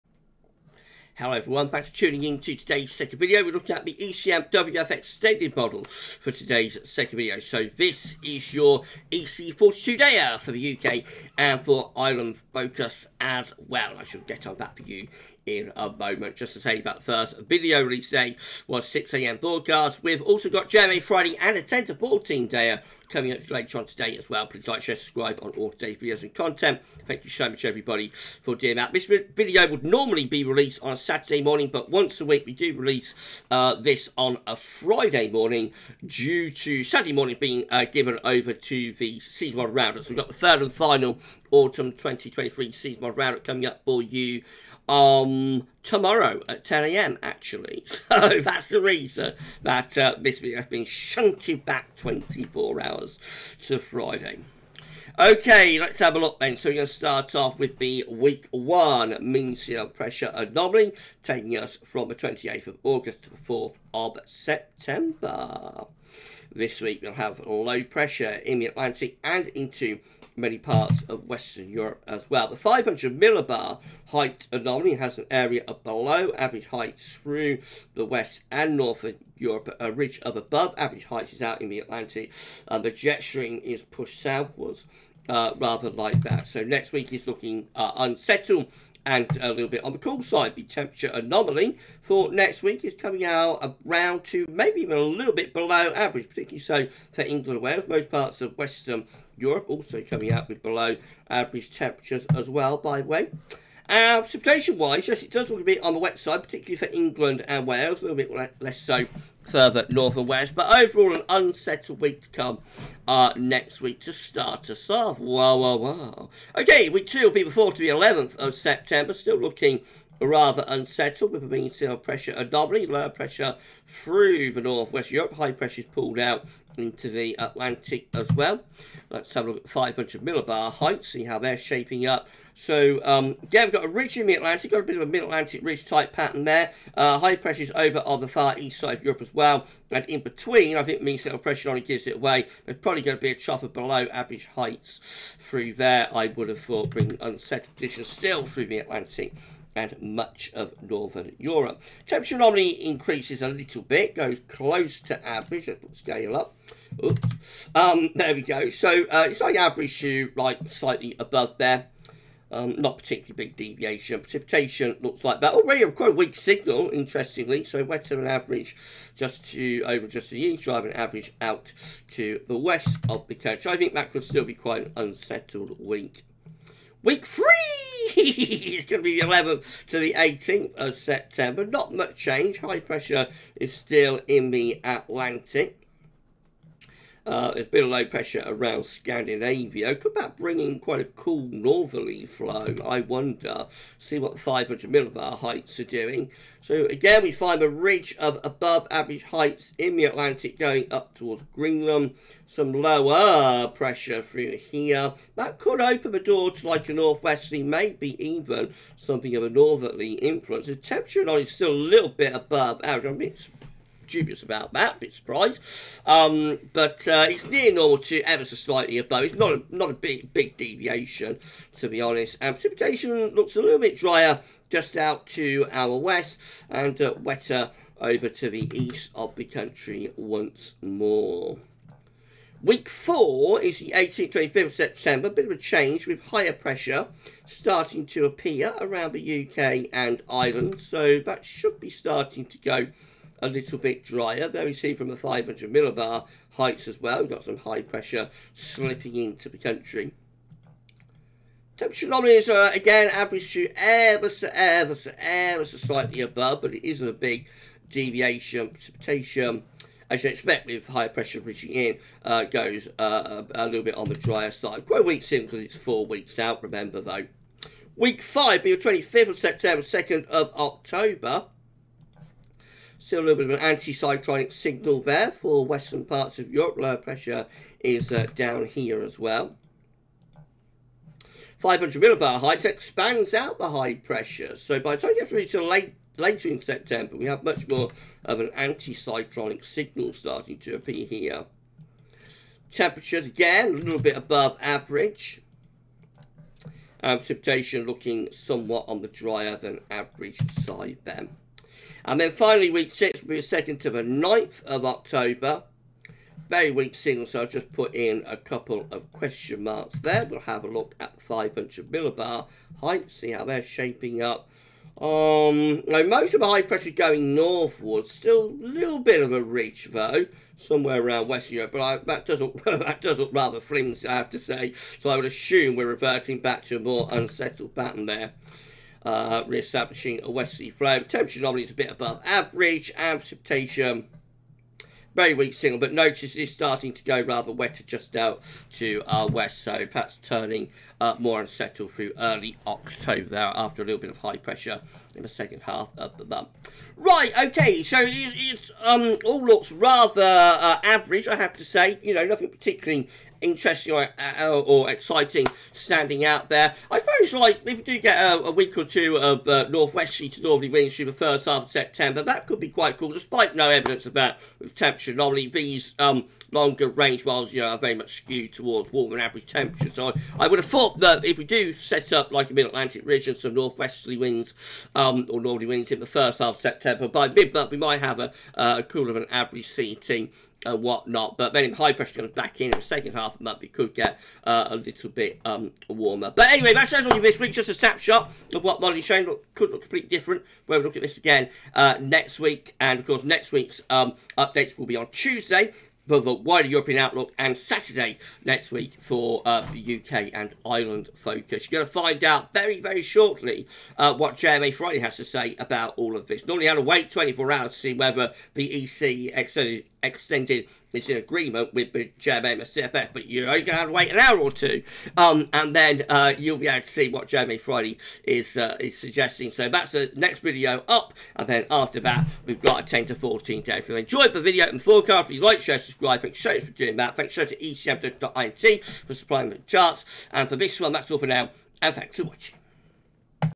ECMWF 42 Day Forecast: Rather Cool September Incoming???
Welcome to this weeks extended ECMWF 42 day forecast for the UK and Ireland.

This video forecast from the extended ECMWF model takes us from 27th August to 9th October 2023 and we're looking to see if a rather cool September could be incoming...

💡  Video Credits: 💡

 💡 GWV Links: 💡